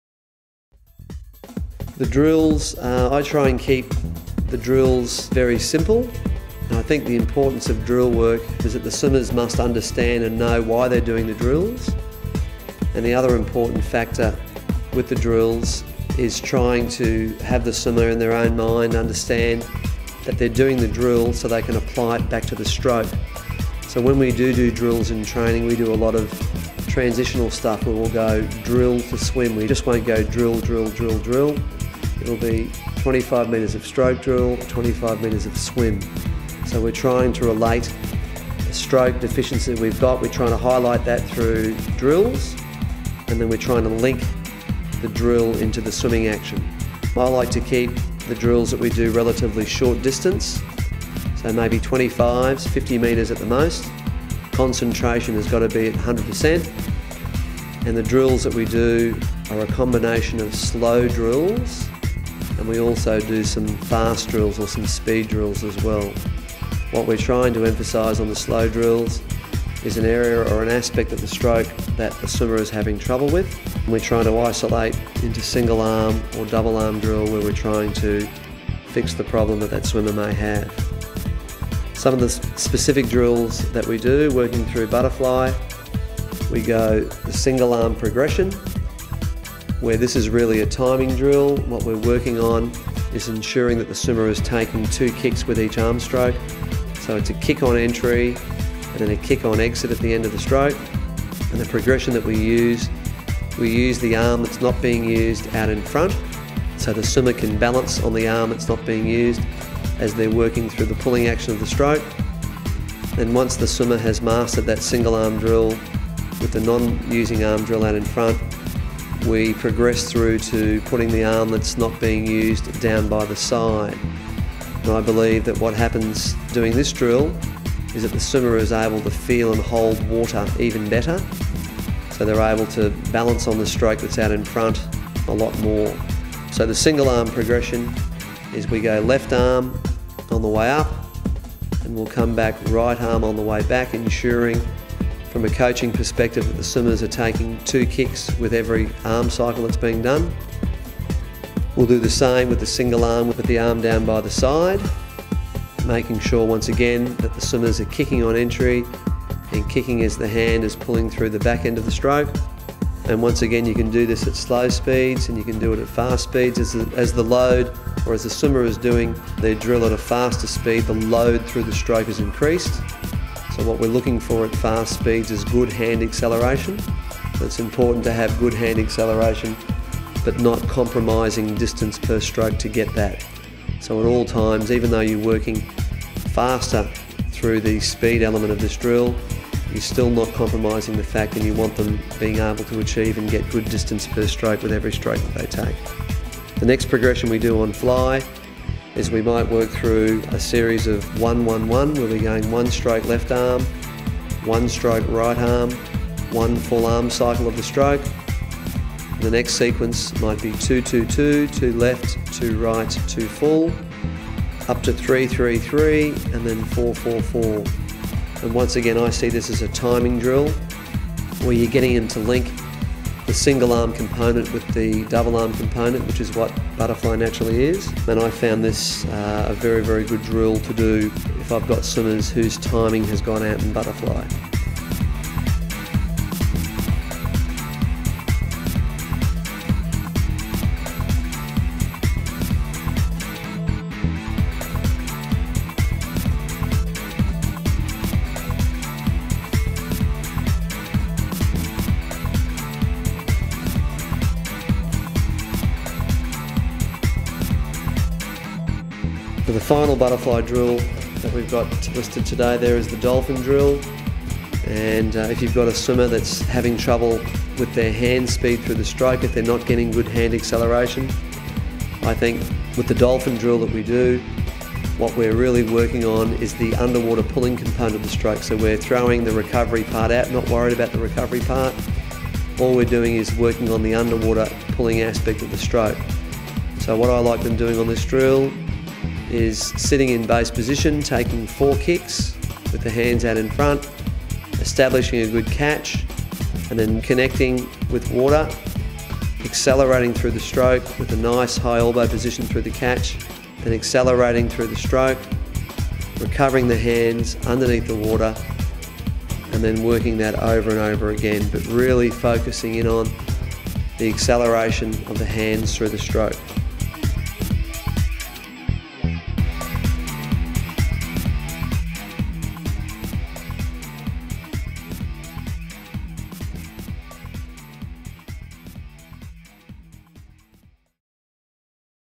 Stephanie Rice Butterfly Workout Video
With commentary by Stephanie's coach Michael Bohl. This drill workout video explains the aims behind the selected drills, and why Michael used them to fine-tune the technique which led to world records.